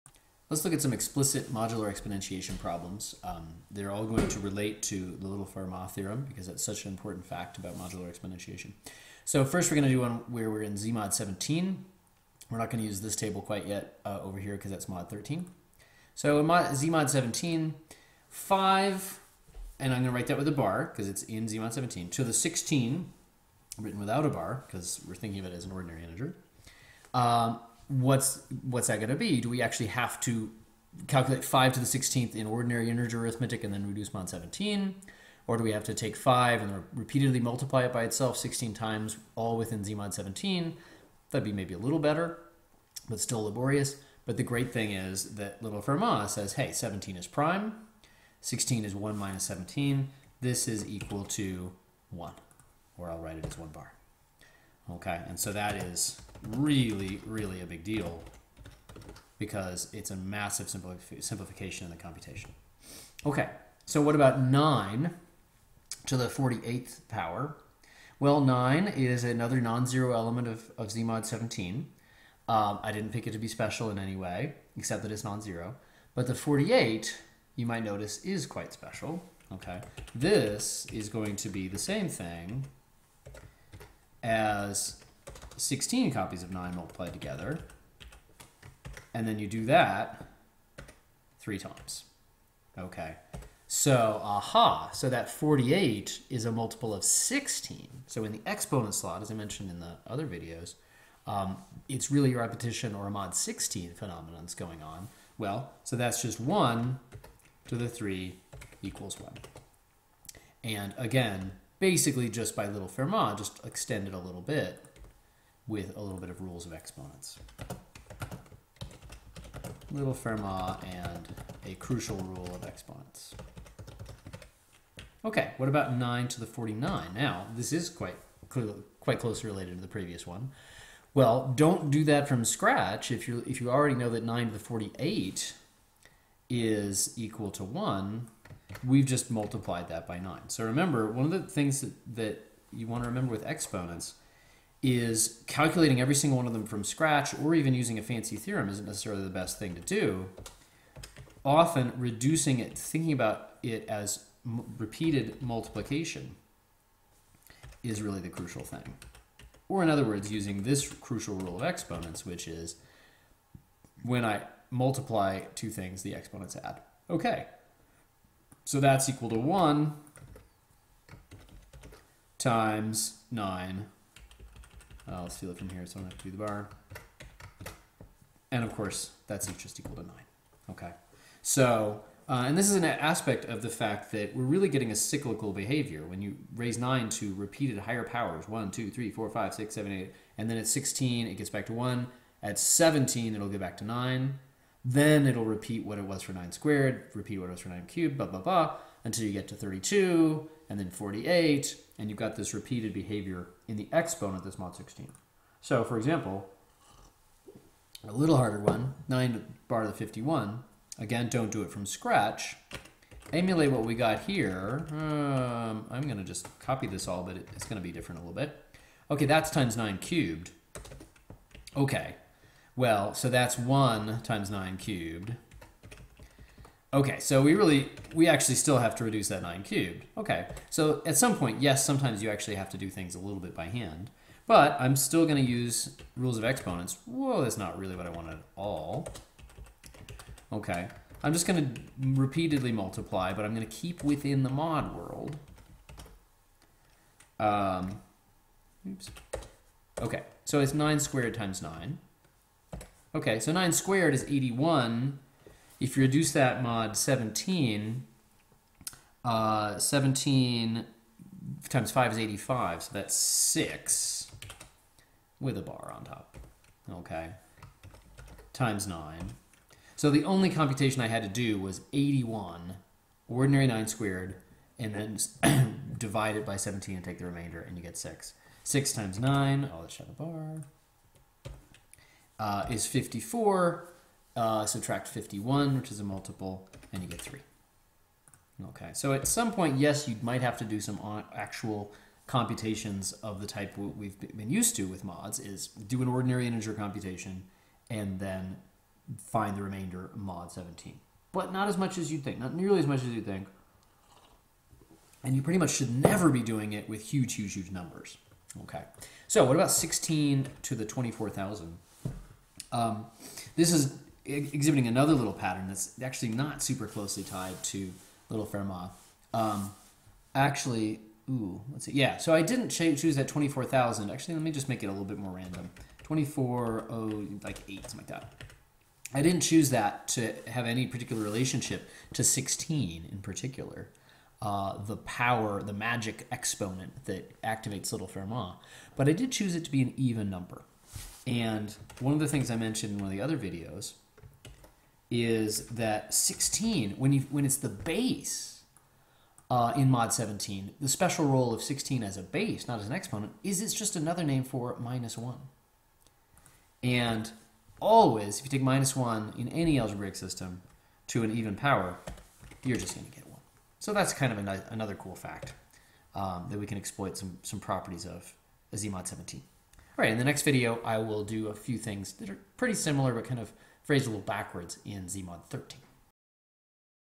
Modular Exponentiation Examples (Part 1)
A two-part video about calculating exponents mod a prime modulus, using Little Fermat as well as basic rules of exponents, and looking for patterns as well.